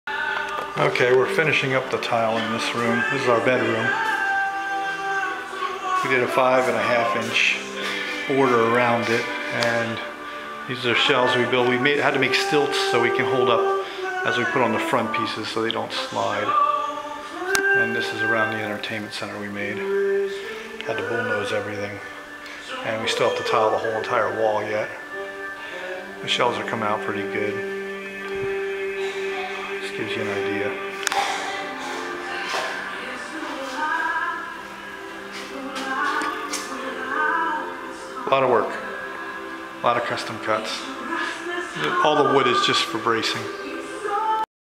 bedroomtile.AVI
Tiling our bedroom in travertine. Since we have down time between last house and 4 plex. I decided to have Rob tile my bedroom and build  custom shelves and an entertainment center. We had to make bullnose as we are to cheap to spend the 6 plus dollars a lin foot for it. All these cuts took a lot longer then we thought. Should be done tomorrow. He took a lot of breaks as I drug him through some properties I wanted to buy.